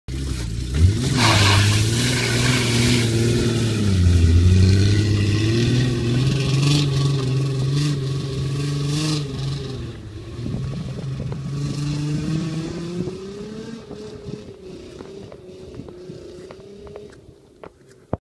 Hill 23%nachylenia Podjazd po delikatnym lodzie audi 80 quattro Gliczarów Tour de Pologne- difflock
nie jest bardzo stromo,lecz z miejsca (bez rozpedzania )czasem nie ma możliwosci ruszyc ponieważ prawa fizyki sciagają samochód w dół ,22-23stopni nachylenia robią swoje,Po sniegu to już żaden problem:)